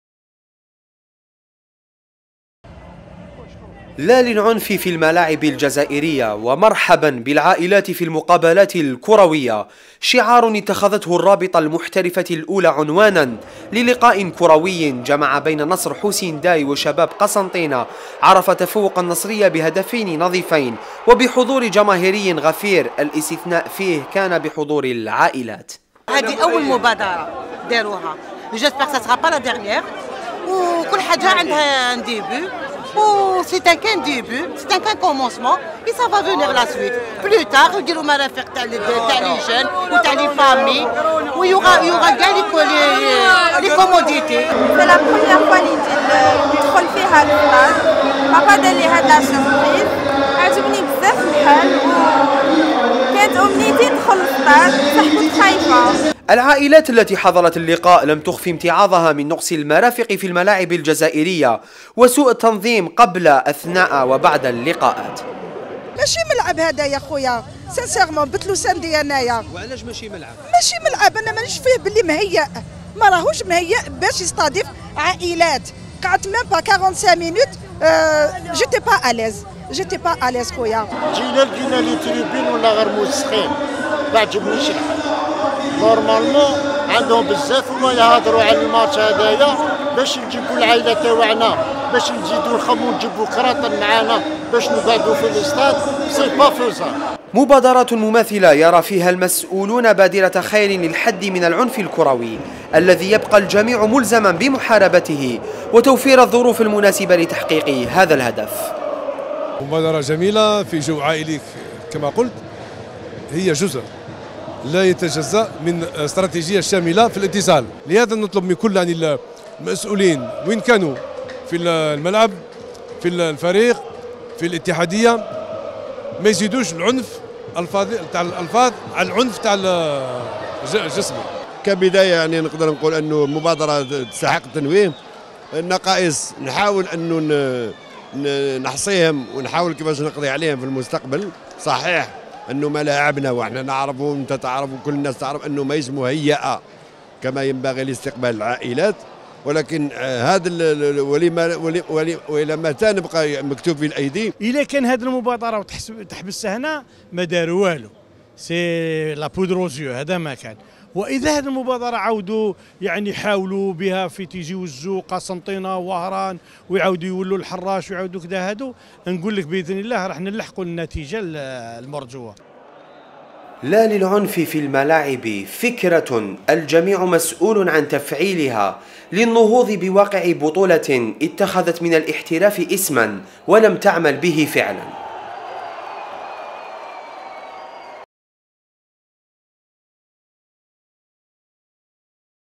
لا للعنف ومرحبا بالعائلات في الملاعب عنوان مباراة النصرية و السنافر
الجزائر (وأج) - تحت شعار لاللعنف و مرحبا بالعائلات في الملاعب الكروية، جرت مباراة النصرية ضد شباب قسنطينة، في إطار الجولة التاسعة من عمر رابطة الدوري الحترف الأول موبيليس، التي انتهت بفوز نصر حسين داي بثنائية أوزناجي ، الإستثناء فيها كان العنصر النسوي في المدرجات،لترسيخ ثقافة اللقاءات الكروية للفرجة و ليست للعنف.